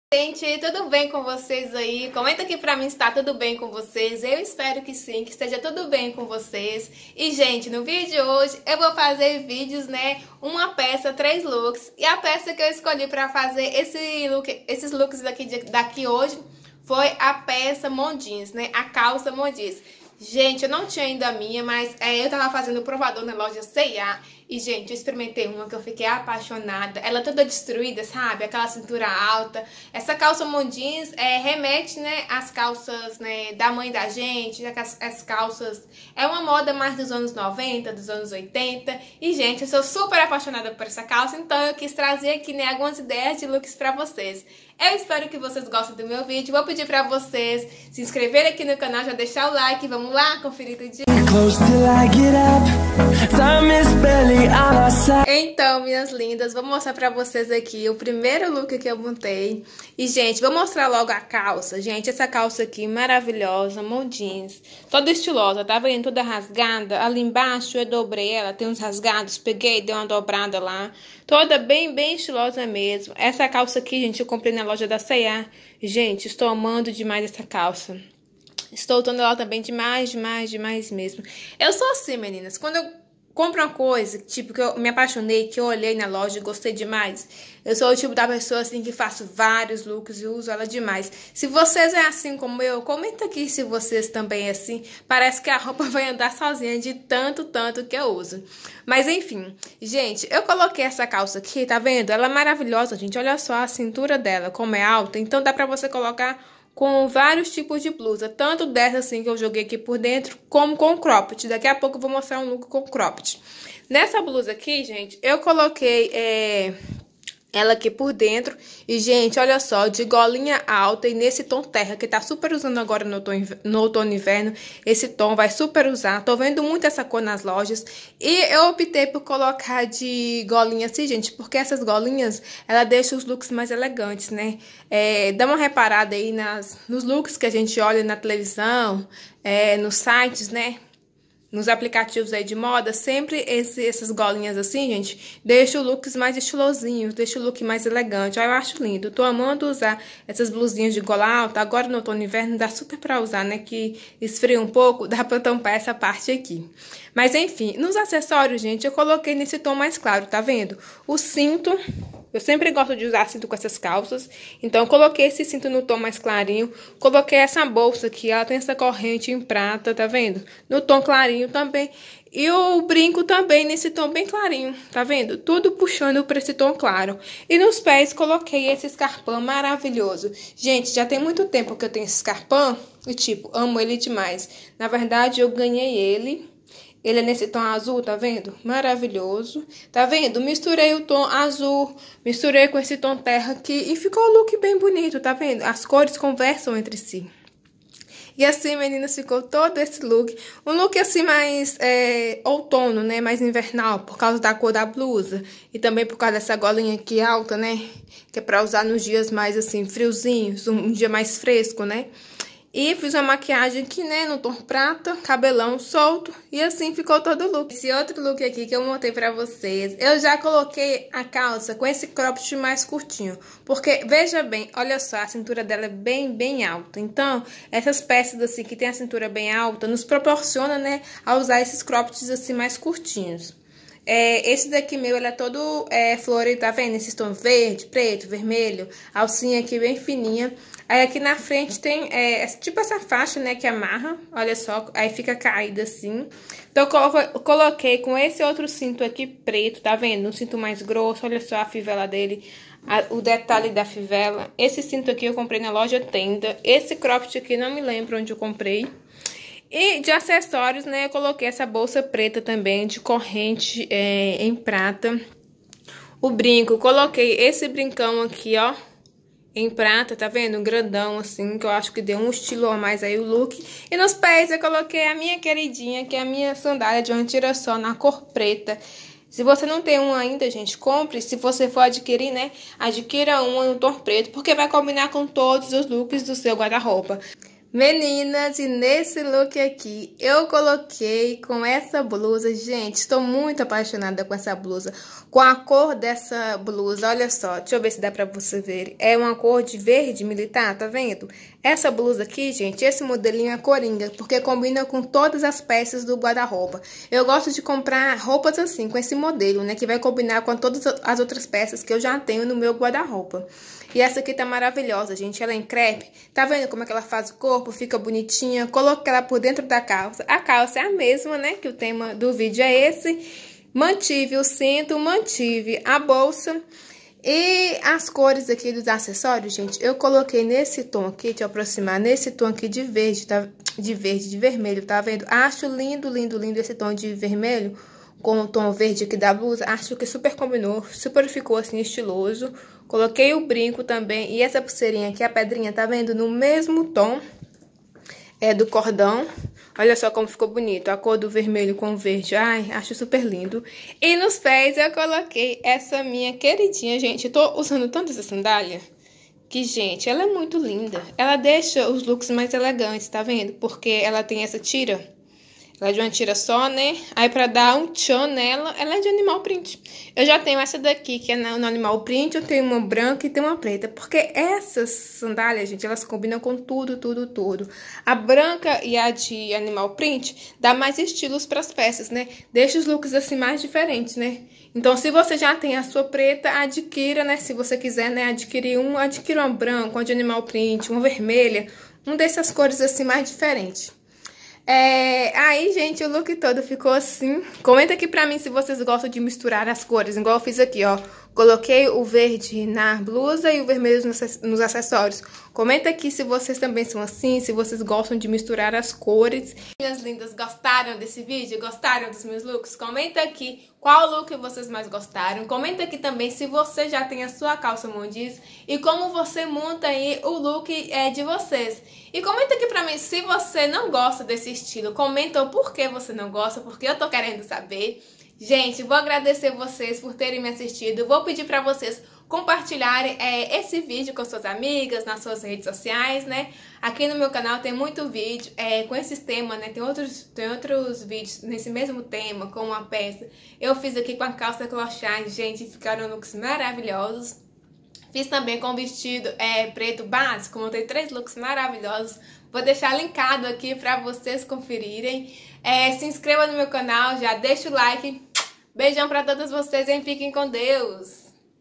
LOOKS CALÇA JEANS- CALÇA MOM JEANS
Oiii Amores  nesse vídeo montei looks com a calça mom jeans, espero que gostem ,inscrevam se no canal!